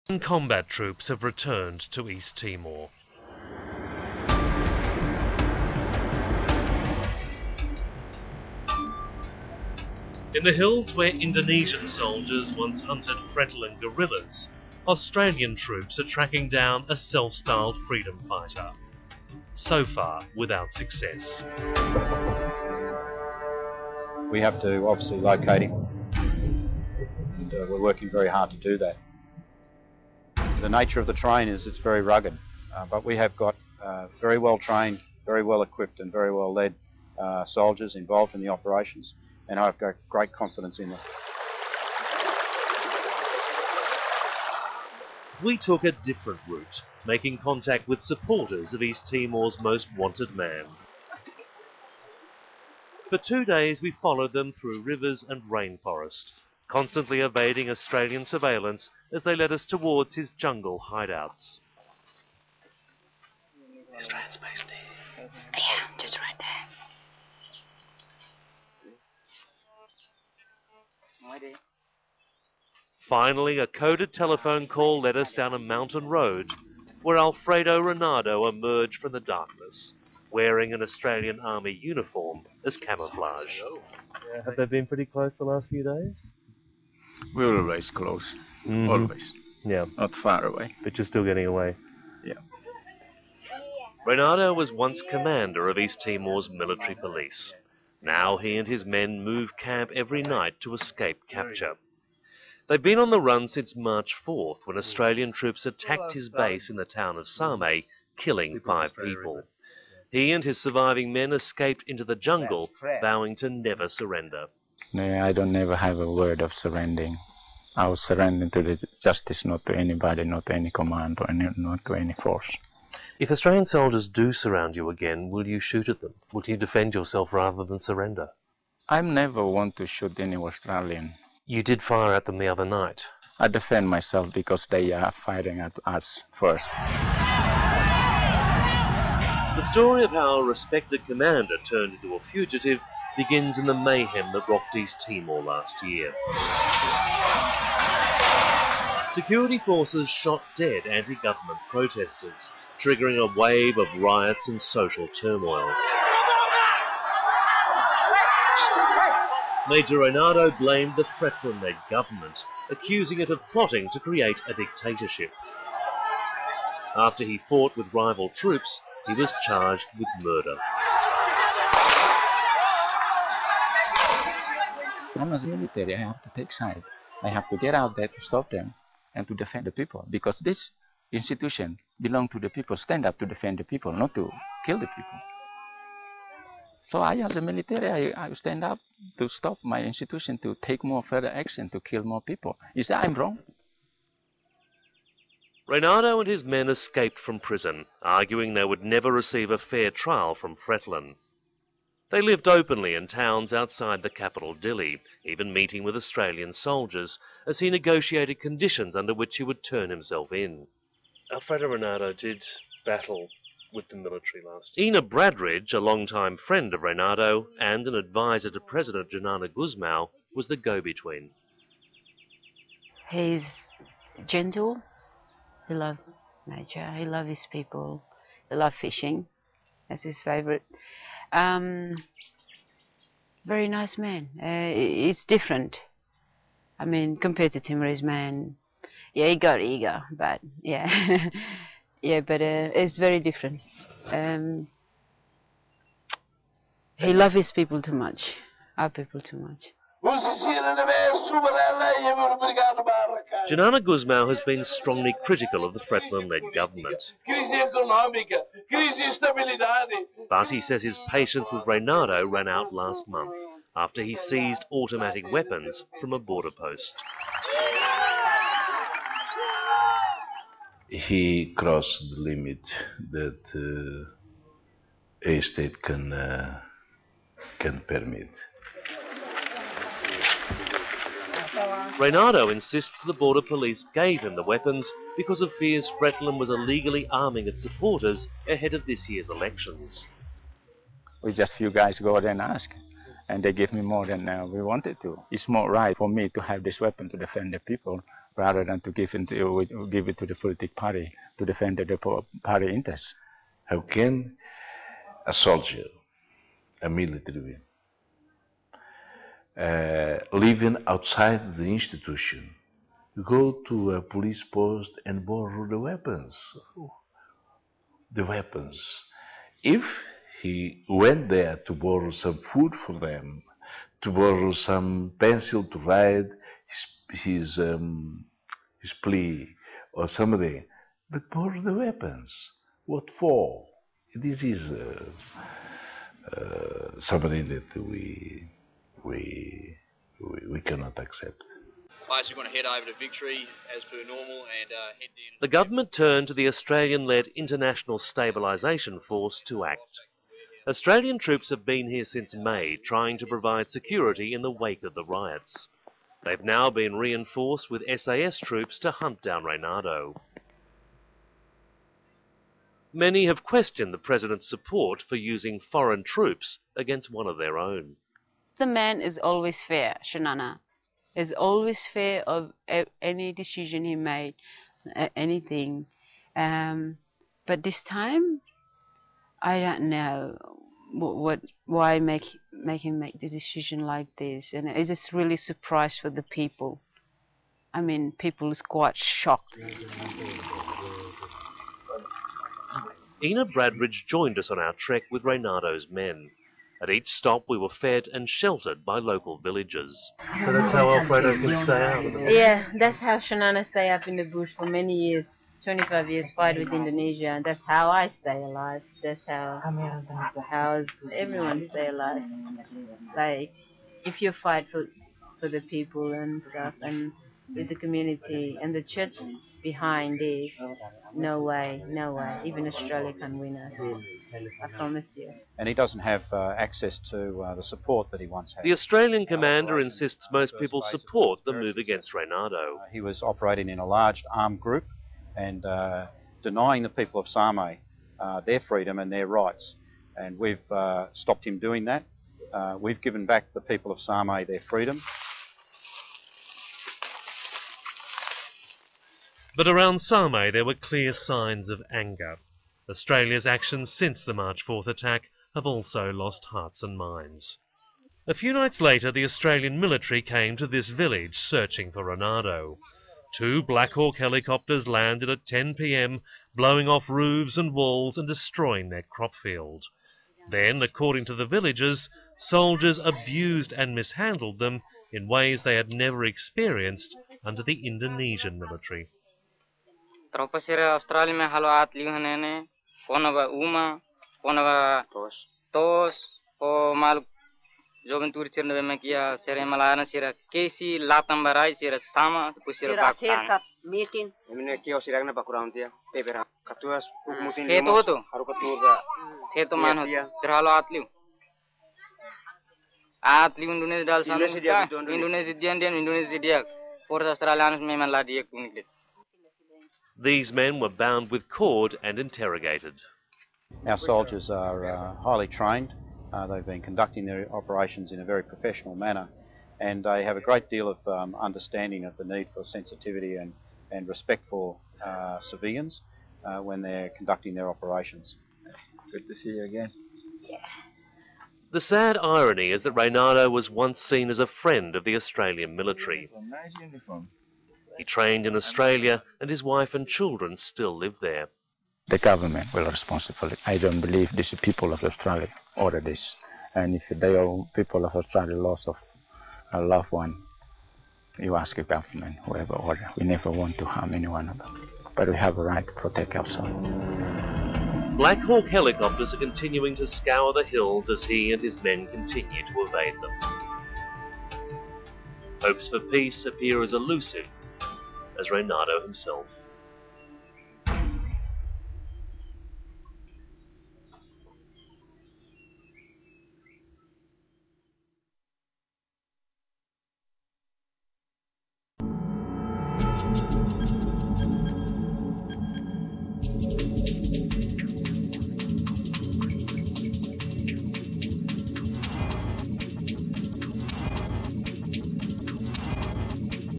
Alfredo Reinado - East Timor
March 2007
Following the death of rebel leader Alfredo Reinado during a failed coup we bring back an exclusive interview and profile of him. In it, he explains why he turned against the government.
Reinado was the respected police commander who turned into East Timor's most wanted man. When security forces shot dead anti-government protestors last March, Reinado attacked rival troops. "I had to get out there and stop them", he reasons. "I had to defend the people". Claiming he would never receive a fair trial, he went on the run.

Produced by ABC Australia
Distributed by Journeyman Pictures